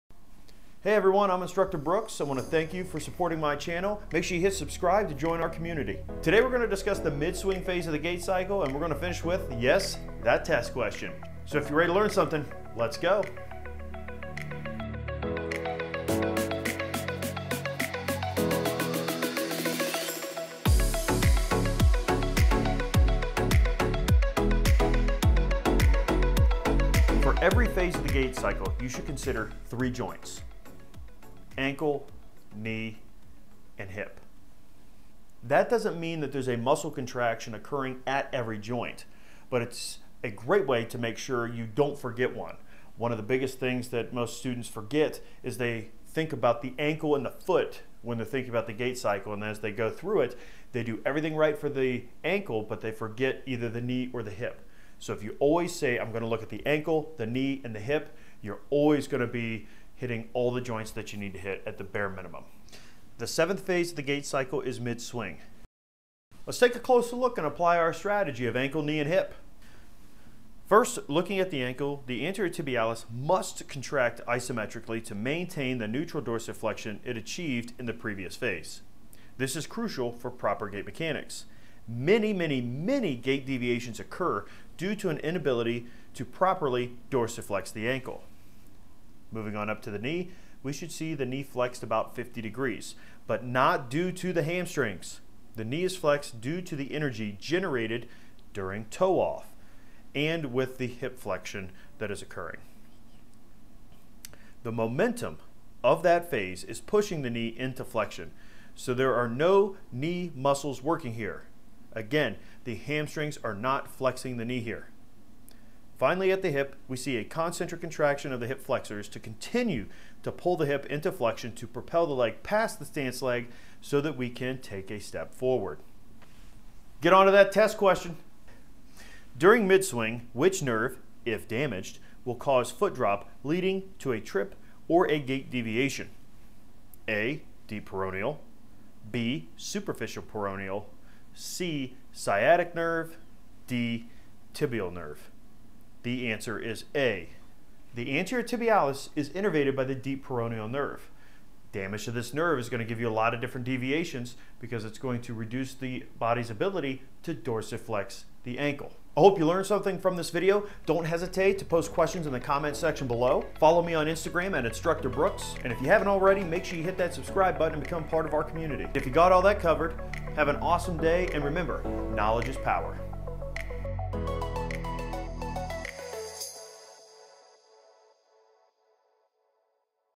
What Happens During Mid Swing?
Are you a physical therapy student who is struggling to understand the gait cycle? Maybe you are looking for/need a better explanation of each individual phase? Perhaps you felt confident when taking your test but ultimately didn’t get the grade you were expecting for the the amount of time you put into studying? 🤯😩

Join me, Instructor Brooks, as I break down the seventh phase of the gait cycle—MID SWING.

In this video, I will break down what is happening at each joint: ankle, knee, and hip. Then I will challenge you with a test question 🚶‍♂️👣

Chapters:
0:00 Introduction
0:27  Analysis Strategy
1:02 Phase 7
1:06 Analyze Mid Swing
2:05 That Test Question ❓

To get access to my learning strategies, quizzes, and more you should check out my entire Instructor Brooks Campus. and click on Campus in the top navigation bar.